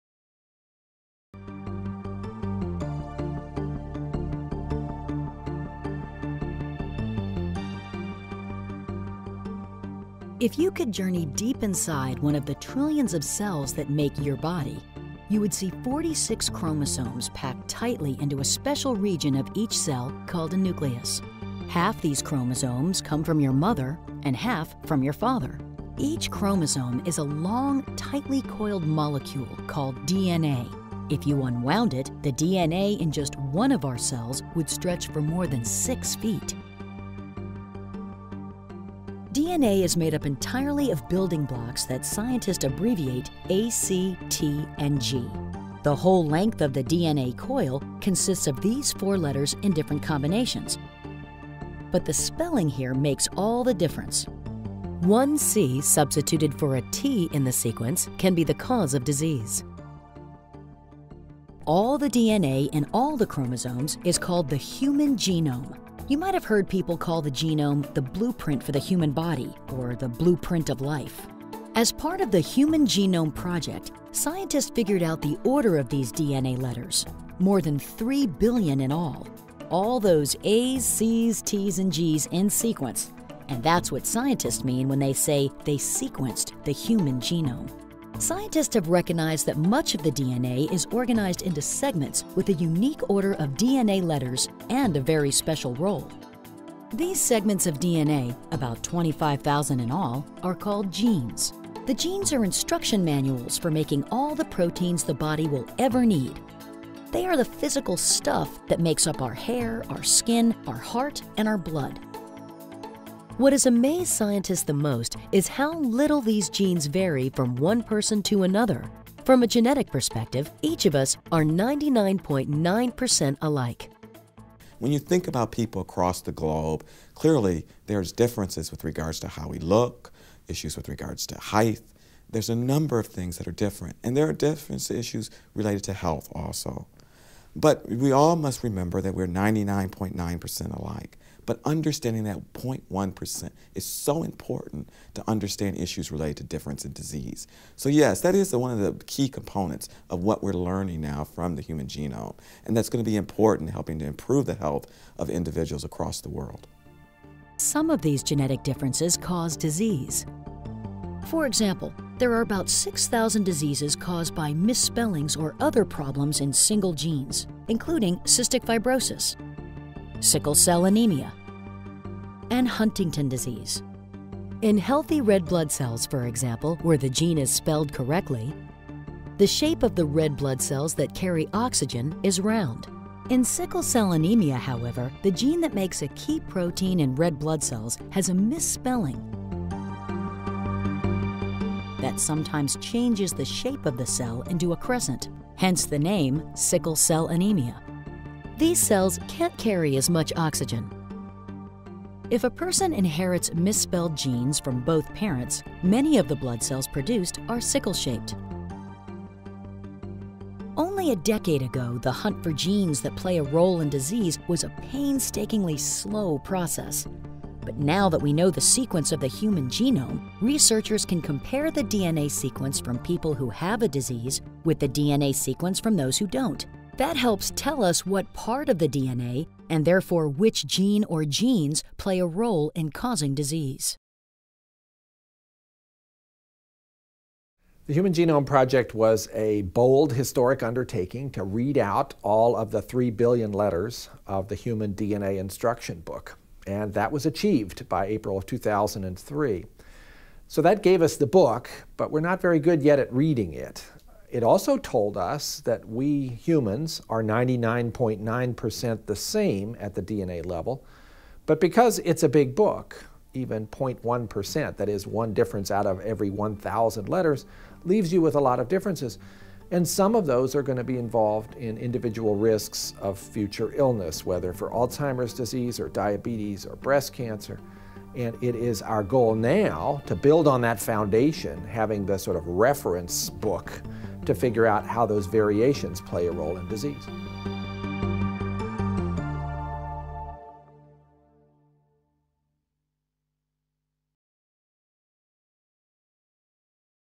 Genes and Health
An overview of the role of genes in shaping health. Made as part of the Genetics and Public Policy Center's Public Consultation on Genetics, Environment, and Health.